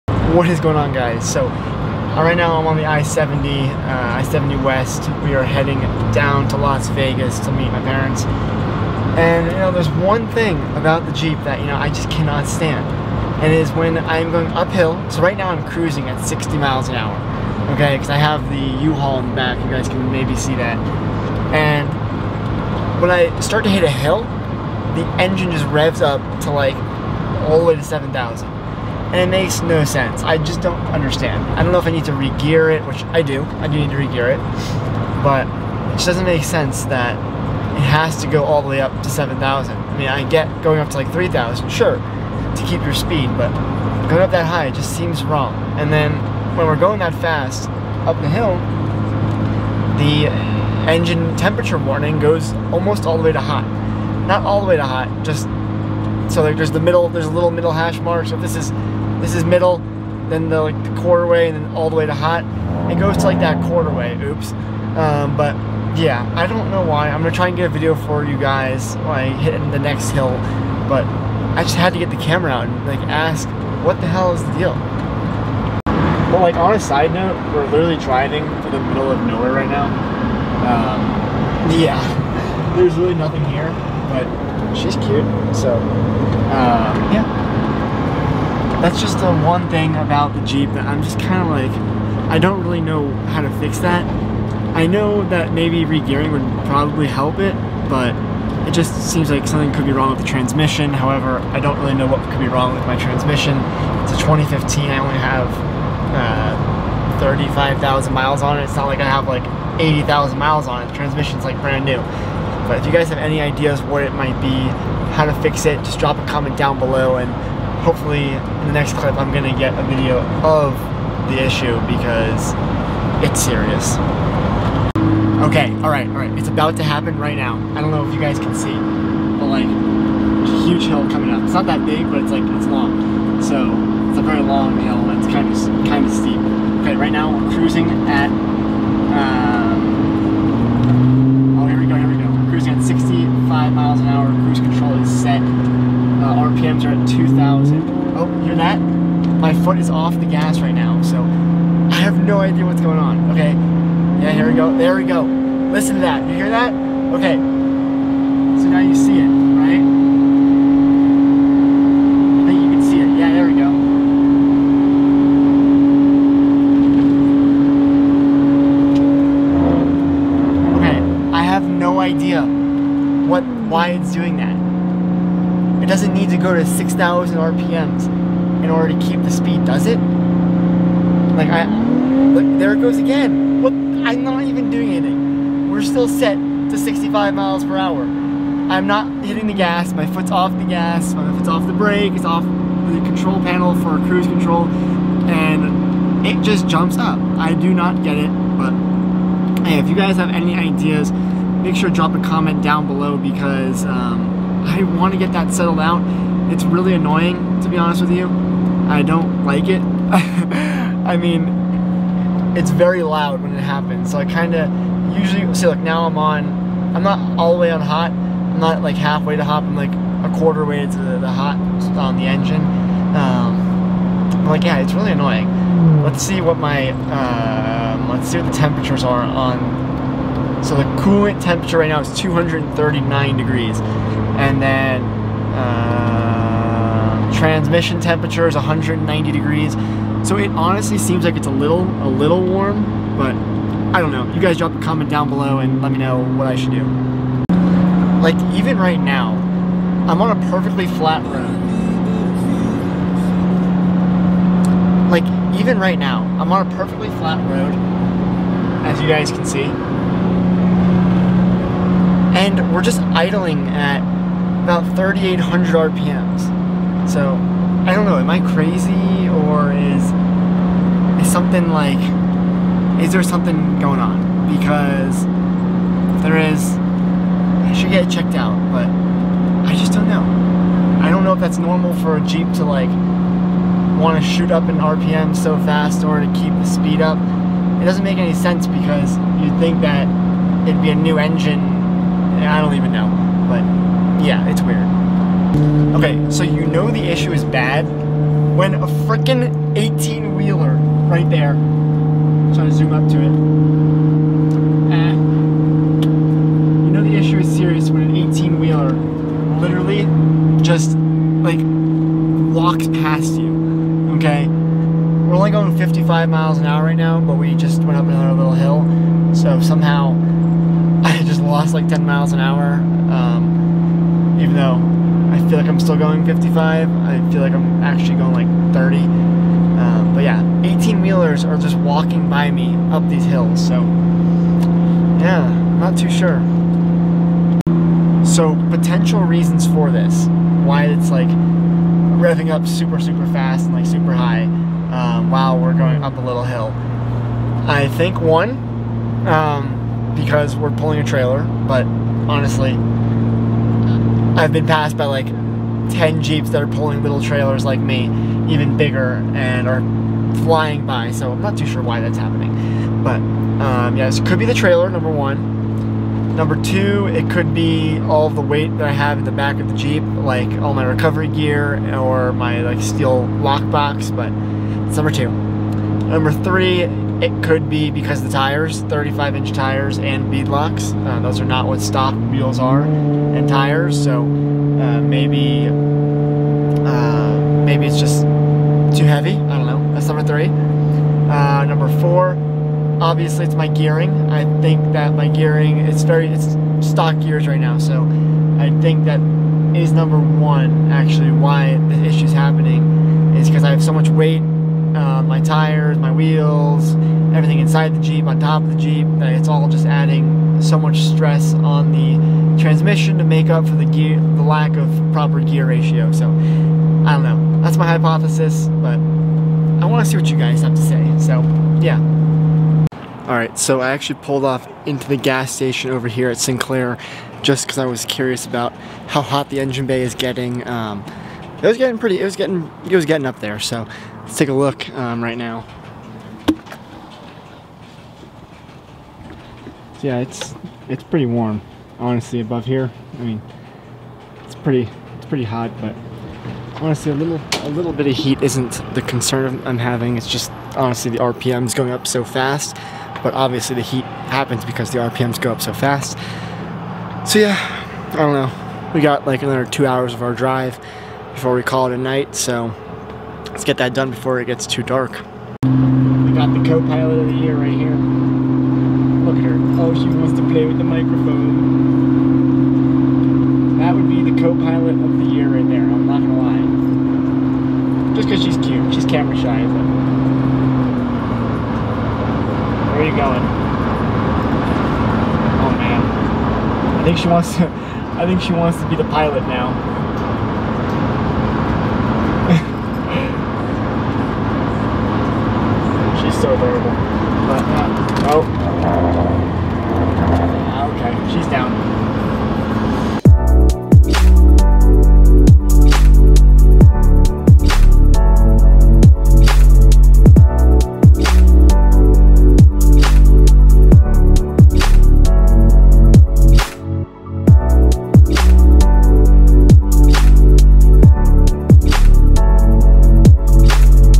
Jeep JK TRANSMISSION Issue - WATCH BEFORE BUYING / MODIFYING A JEEP!
Seems like there could be a small issue with my transmission, however, I'm not too sure. Does anyone have any thoughts on the issue? I think it could be from getting larger/heavier tires and not re-gearing but I'm curious what ya'll think!